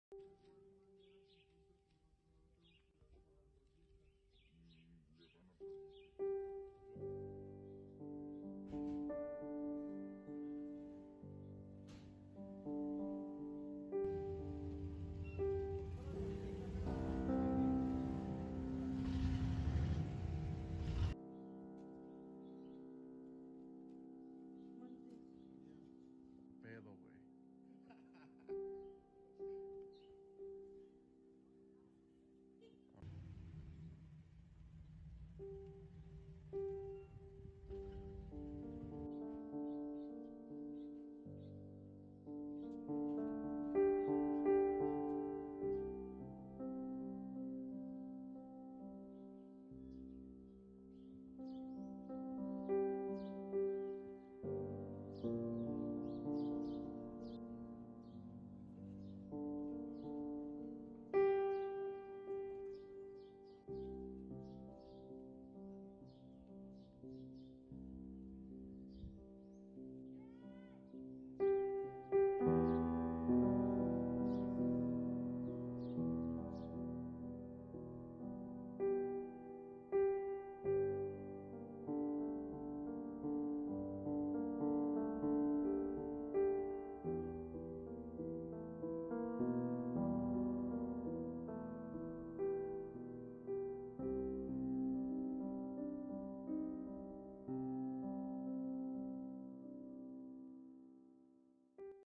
Hallan dos cabezas humanas en Cuernavaca
La mañana de este viernes fueron localizadas dos cabezas humanas en el camino antiguo a Tepoztlán, en el municipio de Cuernavaca.